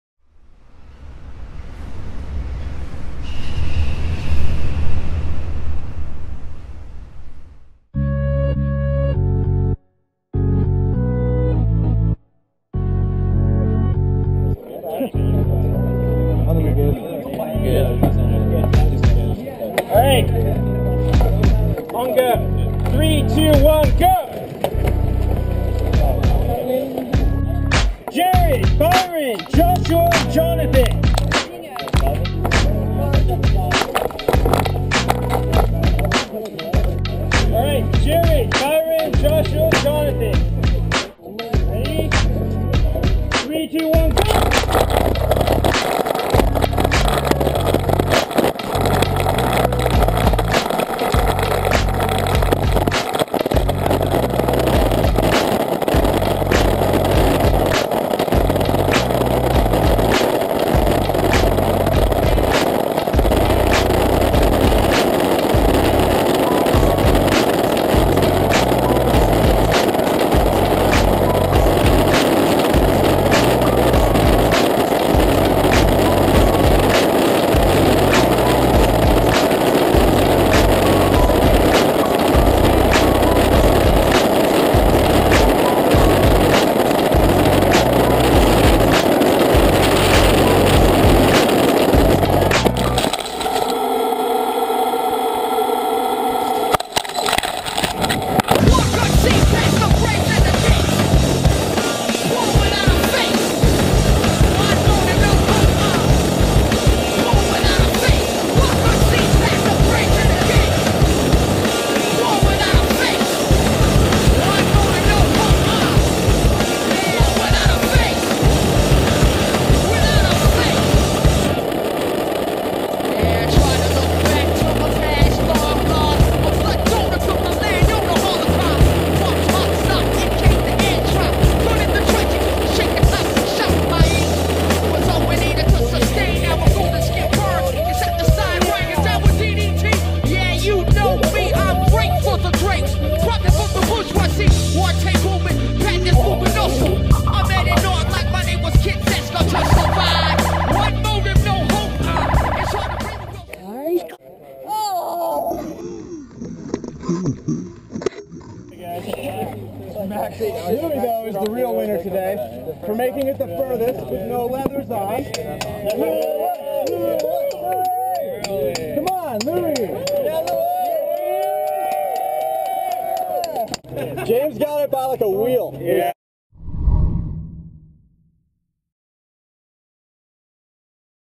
Roscos Wrecker 2013 Trailer
Trailer video for a full Rosco's Wrecker recap coming soon!
Action Sports team rider Jonathan Simon messes up the push, then later crashes when another rider crashes in front of him, but then still hunts down second place to advance to the next round!

Featured:
Daniel Vega
Peter Johnson
Ryan Bishop
Jonathan Simon